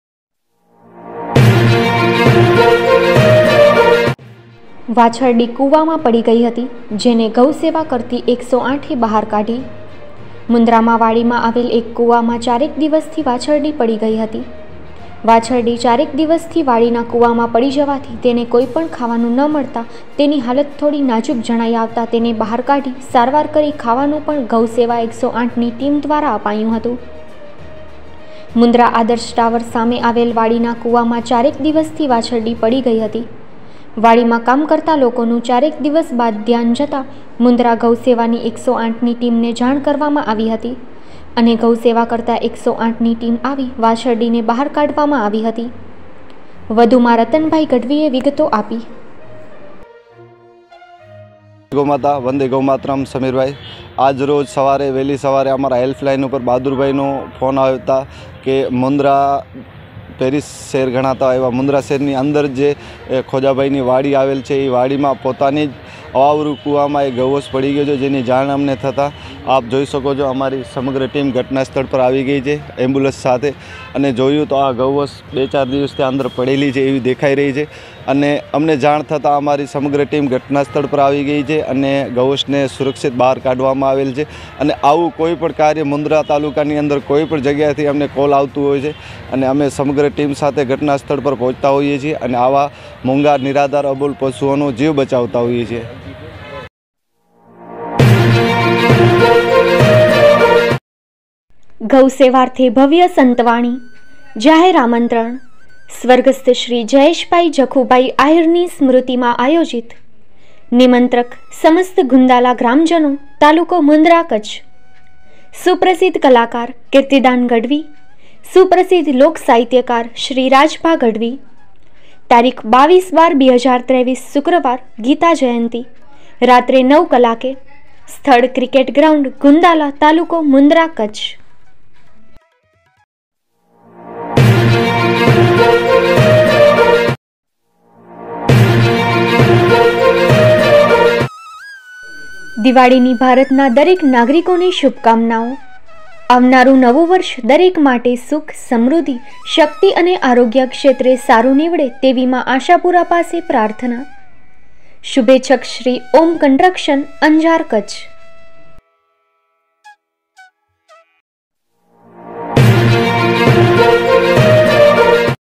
મુન્દ્રામાં વાછરડી કુવામાં પડી ગઈ હતી જેને ગૌ સેવા કરતી 108'એ બહાર કાઢી...
મુંદ્રામાં વાડીમાં આવેલ એક કુવામાં ચારેક દિવસથી વાછરડી પડી ગઈ હતી. વાછરડી ચારેક દિવસથી વાડીના કુવા પડી જવાથી અને તેને કાઈ પણ ખાવાનું ન મળતાં તેની હાલત થોડી નાજુક જણાઈ આવતા તેને બહાર કાઢી સારવાર કરી ખાવાનું પણ ગૌ સેવા 108 ની ટીમ દ્વારા અપાયું હતું...

મુન્દ્રા આદર્શ ટાવર સામે આવેલ વાડીના કુવામાં ચારેક દિવસથી વાછરડી પડી ગઇ હતી વાડીમાં કામ કરતા લોકોનું ચારેક દિવસ બાદ ધ્યાન જતા મુન્દ્રા ગૌ સેવાની 108ની ટીમને જાણ કરવામાં આવી હતી અને ગૌ સેવા કરતા 108 આવીને વાછરડીને બહાર કાઢવામાં આવી હતી. વધુ રતનભાઇ ગઢવીએ વિગતો આપી હતી.